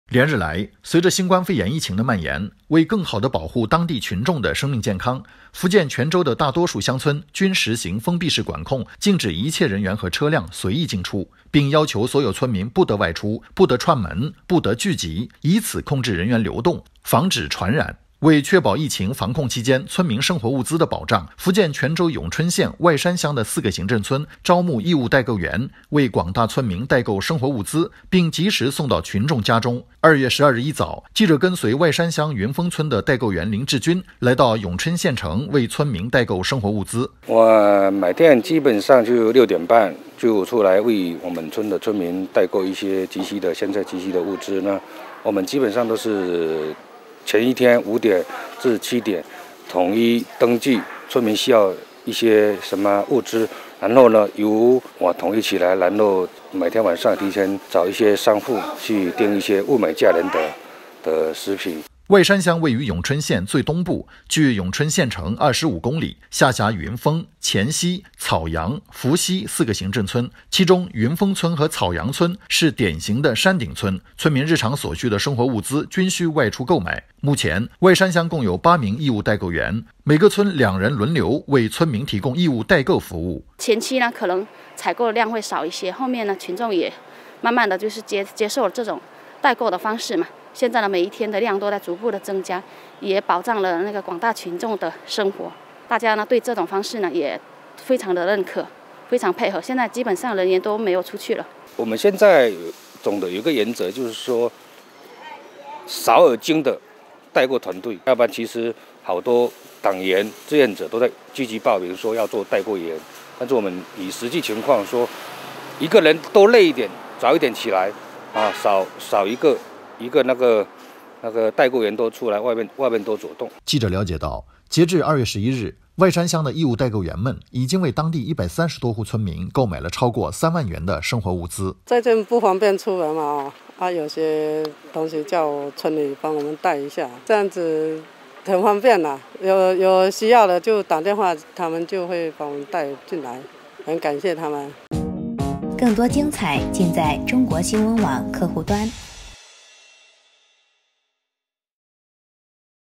福建泉州：义务代购员“解忧”封村生活
连日来，随着新冠肺炎疫情的漫延，为更好地保护当地群众的生命健康，福建泉州的大多数乡村均实行封闭式管控，禁止一切人员和车辆随意进出，并要求所有村民不得外出、不得串门、不得聚集，以此控制人员流动、防止传染。